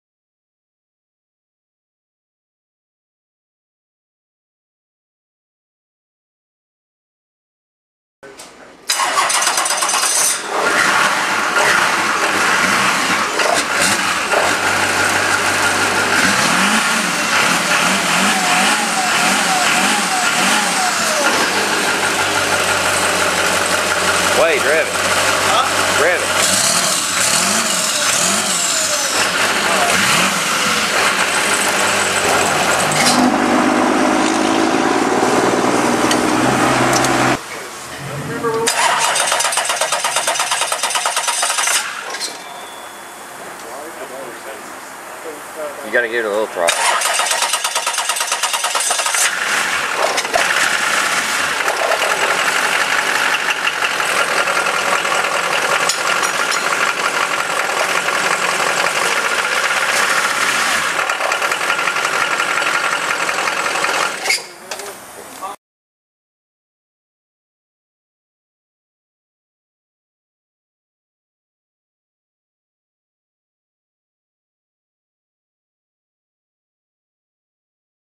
Biodiesel
For our school science fair me and a friend made biodeisel, and to test whether or not it worked we ran it through a 6.6 Duramax diesel our school has. We were all excited and very much relieved that the fuel ran as smooth as it did. We tested i before running it in the engine, and it is actully a lot better at lubricating the engine than regular diesel.

The three things we used to make the diesel with were:
Lye
HEET (methanol)
1 liter of used vegetable

THANK YOU to everyone who helped make this a success!